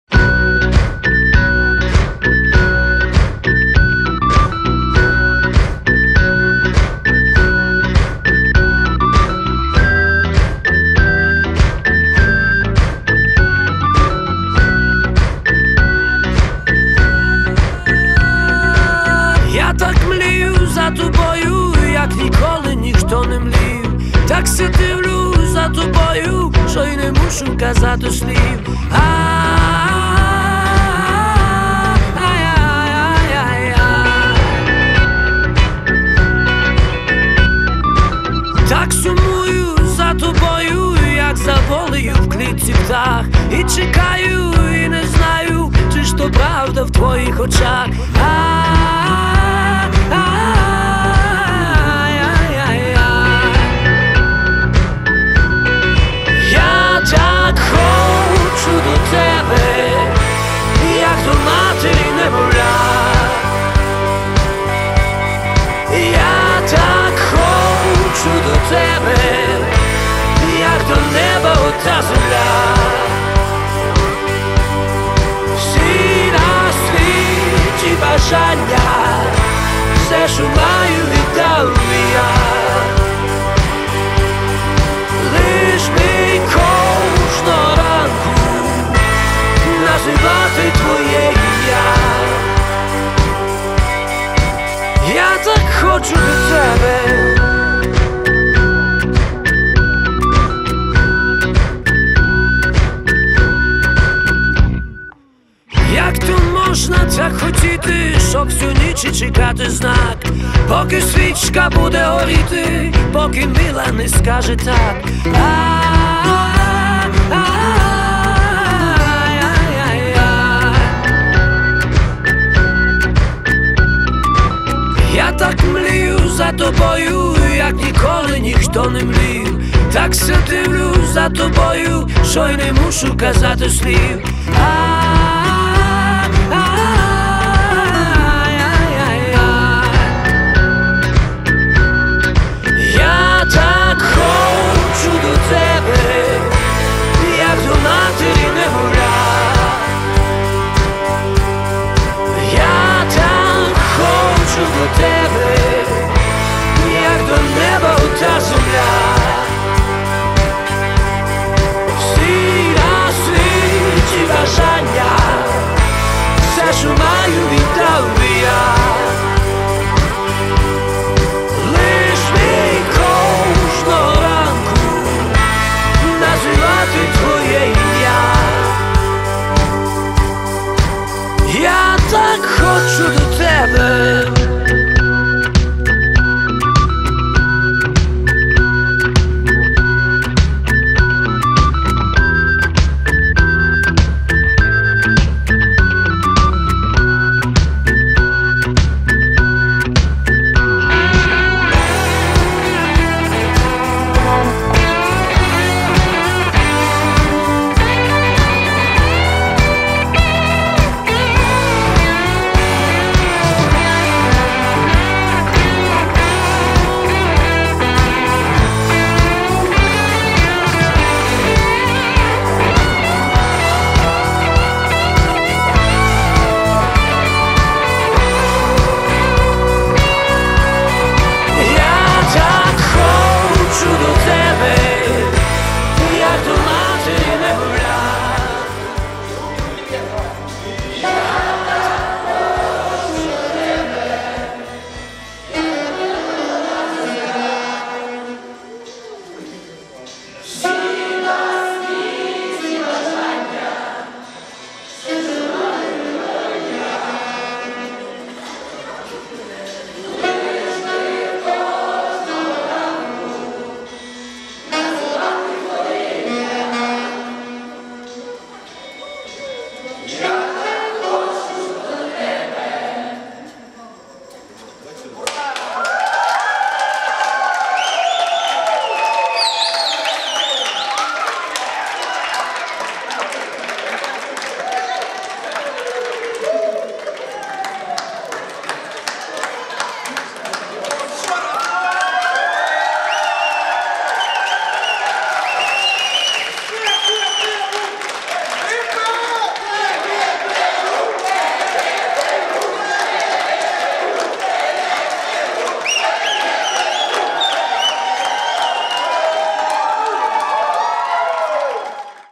LipDub ПГТУ.mpg
15 апреля 2011 года студенты и сотрудники ПГТУ сняли клип в стиле lipdub за 1 час 15 мин